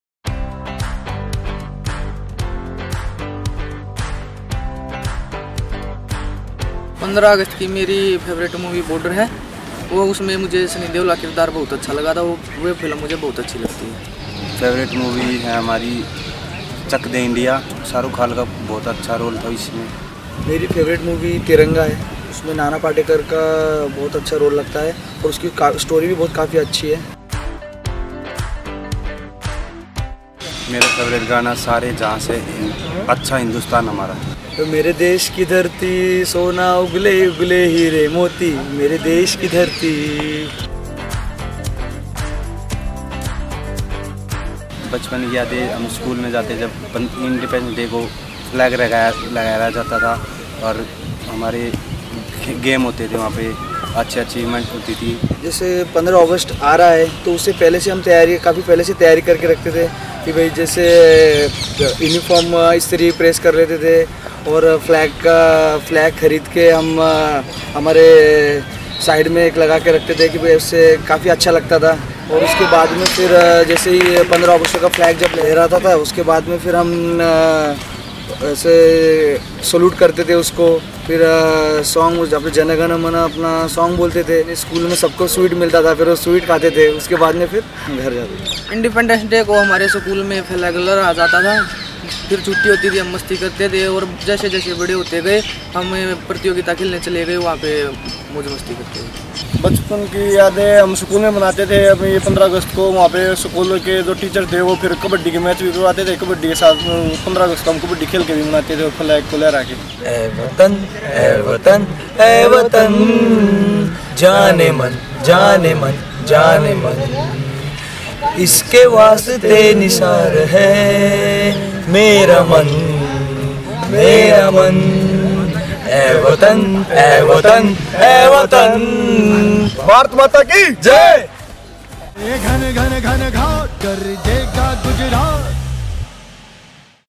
Independence Day
Our Giants recall on their favourite Independence Day memory!